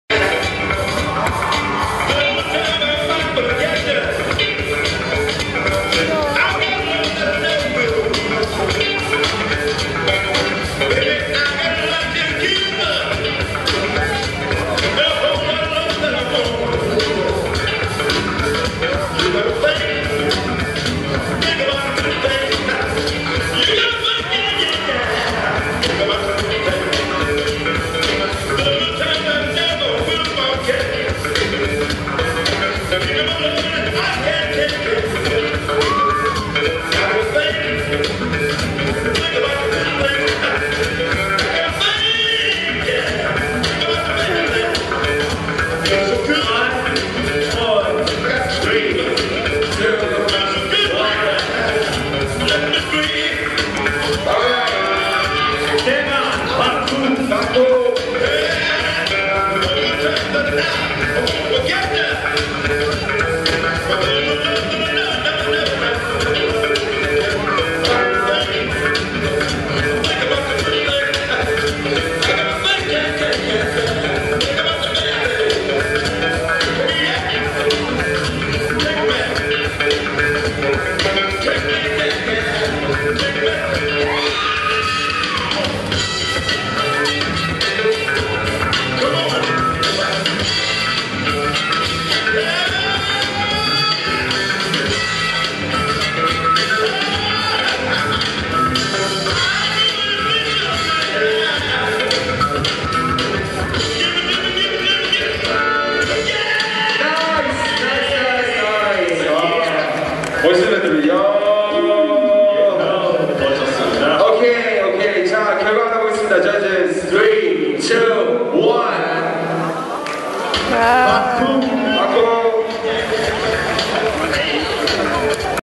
예선 (LOCKING SOLO)_ Baku VS
예선 (LOCKING SOLO)
Baku (Taiwan) VS

20110702 R-16 KOREA 2011
WORLD B-BOY MASTERS CHAMPIONSHIP
DAY1_ SOLO BATTLE (B-BOY, POPPING, LOCKING)
@OLYMPIC HALL